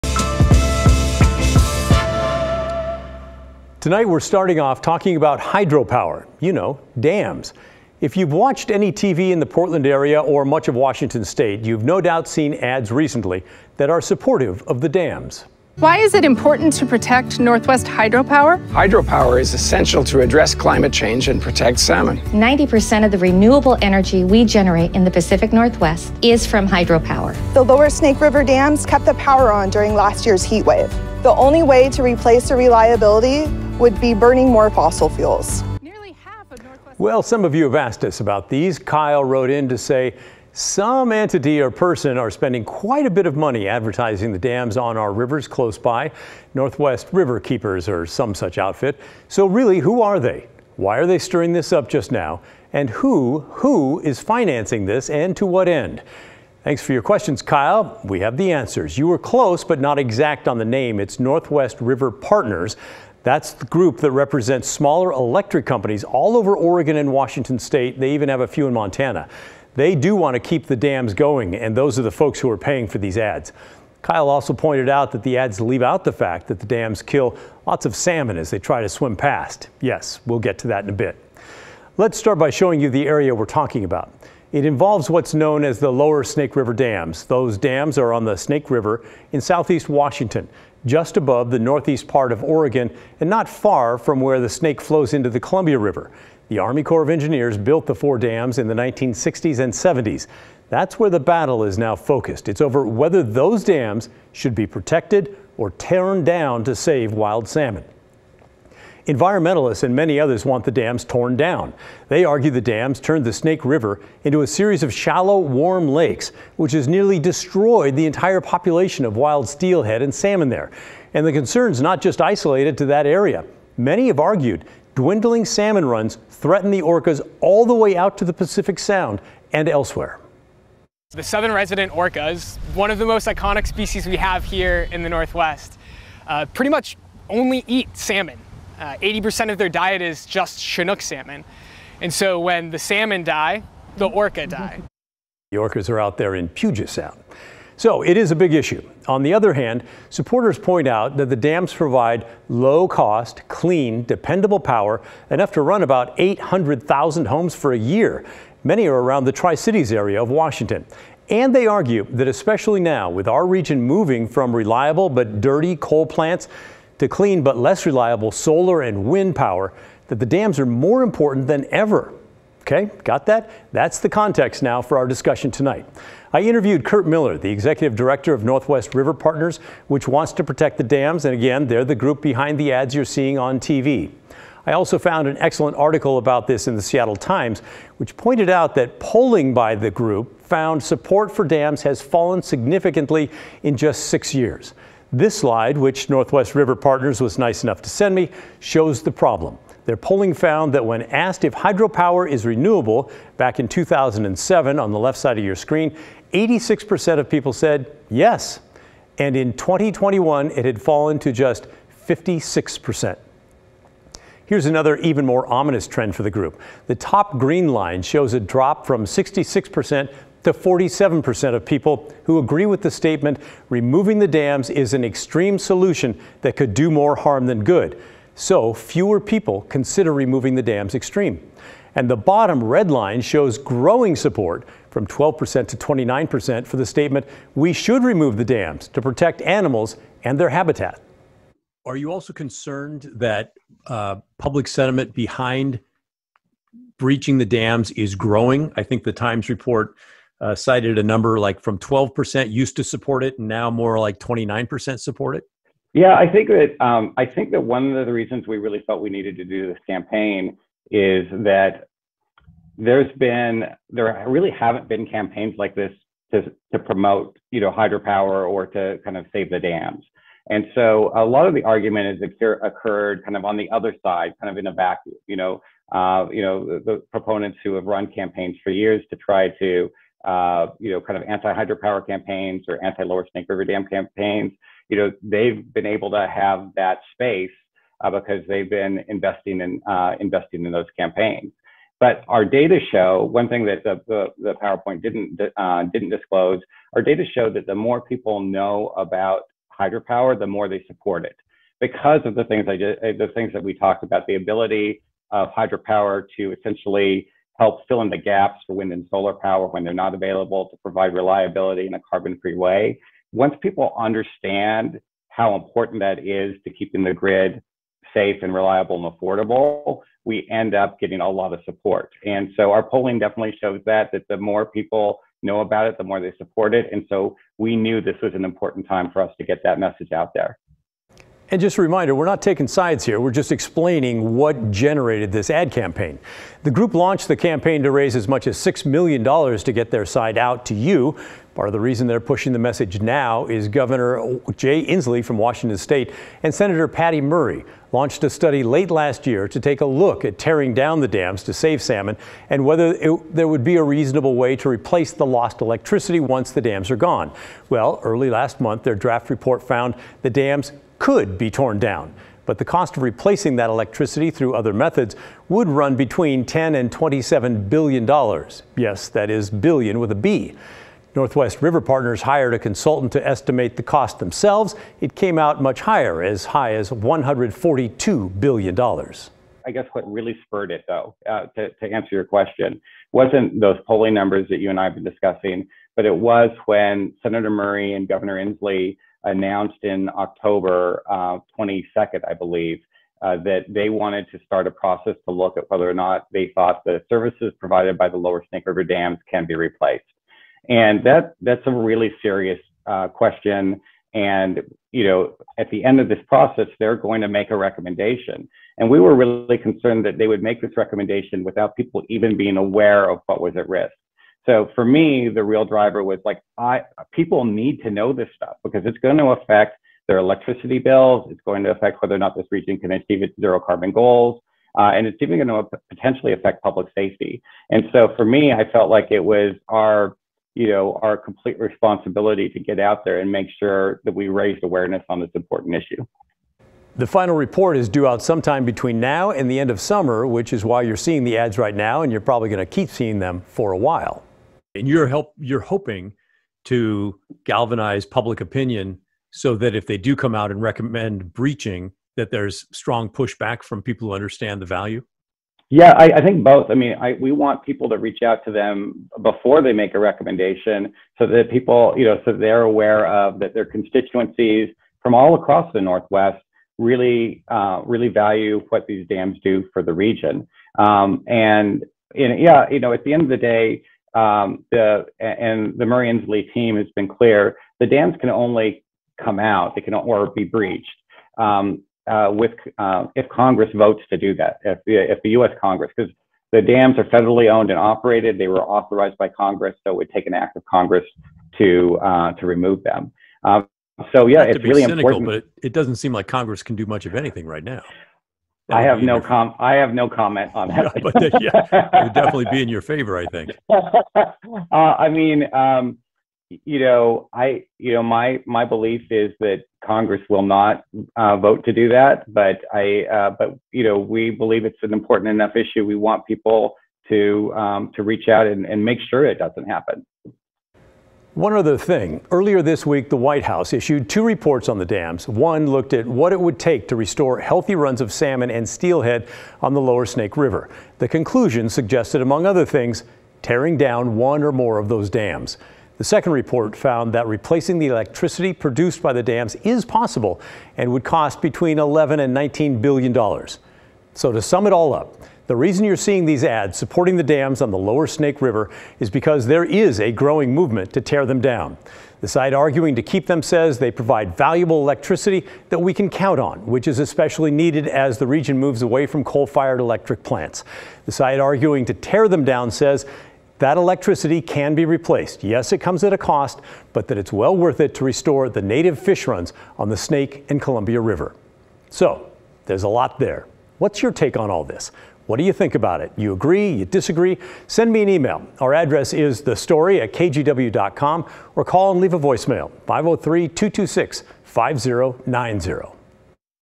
Should the Snake River dams stay or go?
Environmentalists want them removed to protect fish habitats, but electric companies that run them are trying to build support to keep the dams.